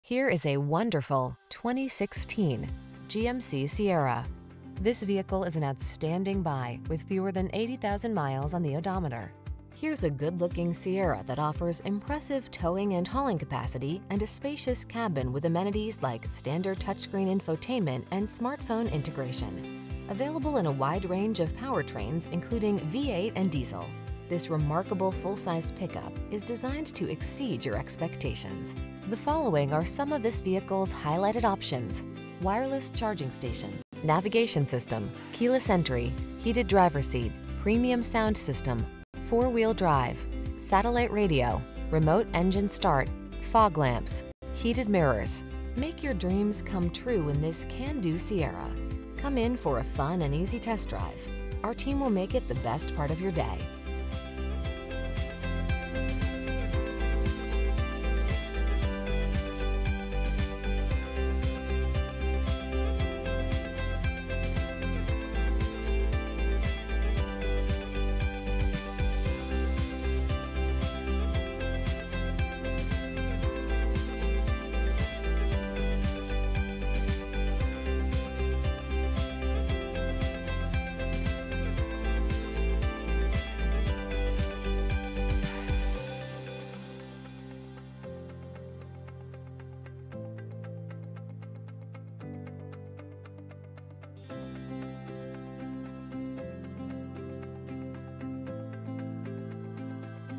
2016 GMC Sierra_1500 SLE OH Troy, Dayton, Tipp City, Piqua, Casstown
Pre-Owned 2016 GMC Sierra_1500 SLE available at Dave Arbogast Buick GMC located in 3540 S Co Rd 25A, Troy, OH, 45373. Servicing the Troy, Dayton, Tipp City, Piqua, Casstown area.
Please mention the stock G17441AT when you contact the dealership for this vehicle.

Here is a wonderful   2016  GMC Sierra.  Here's a good-looking Sierra that offers impressive towing and hauling capacity, and a spacious cabin with amenities like standard touchscreen infotainment and smartphone integration. Available in a wide range of powertrains including V8 and diesel, this remarkable full-size pickup is designed to exceed your expectations. The following are some of this vehicles highlighted options: Wireless Charging Station, Navigation System, Keyless Entry, Heated Driver Seat, Premium Sound System, Four Wheel Drive, Satellite Radio, Remote Engine Start, Fog Lamps, Heated Mirrors. Make your dreams come true in this can-do Sierra. Come in for a fun and easy test drive. Our team will make it the best part of your day.